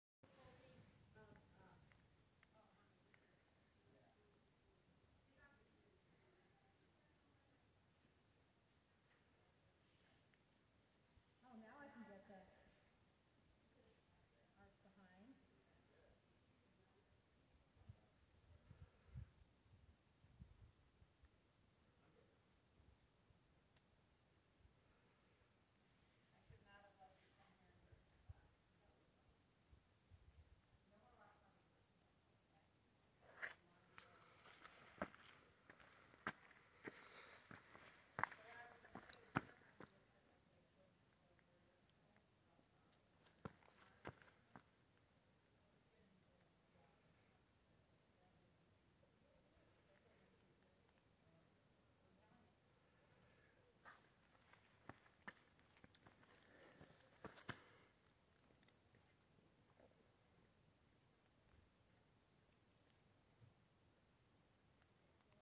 Near 'The Windows' at Arches National Park
Including nearby LaSal Mountain Range with a fresh coat of snow received the night before. Recorded 3/30/2022.